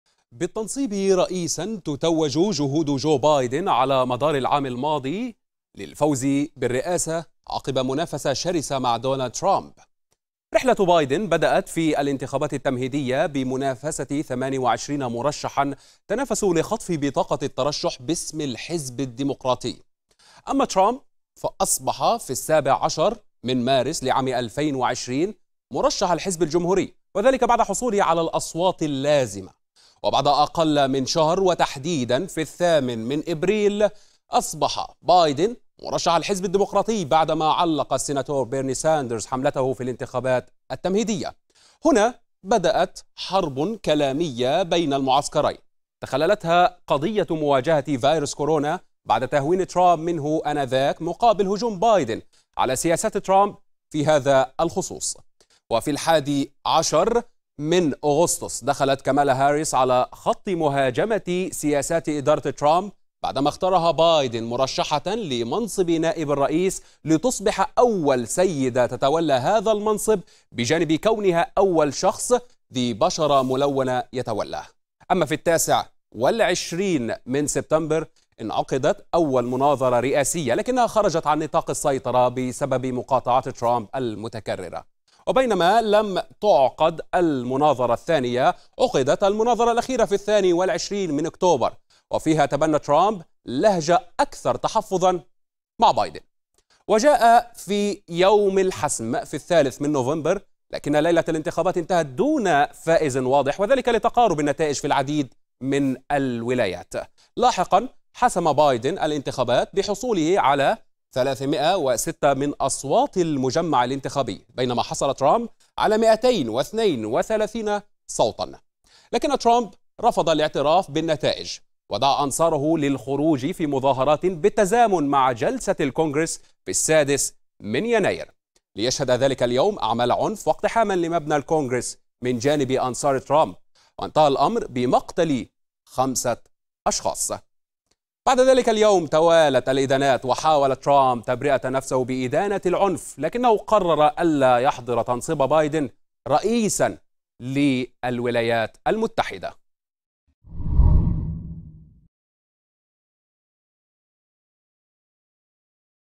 أبزر محطات السباق في الانتخابات الأمريكية
أسدل الستار إذن على ولاية ترامب وهي الفترة التي وصفت بالعصيبة في تاريخ الولايات المتحدة وجاء الرئيس المنتخب جو بايدن بعد سباق محموم إلى البيت الأبيض.. أبرز محطات السباق مع الإعلامي صلاح الدين الوحيدي.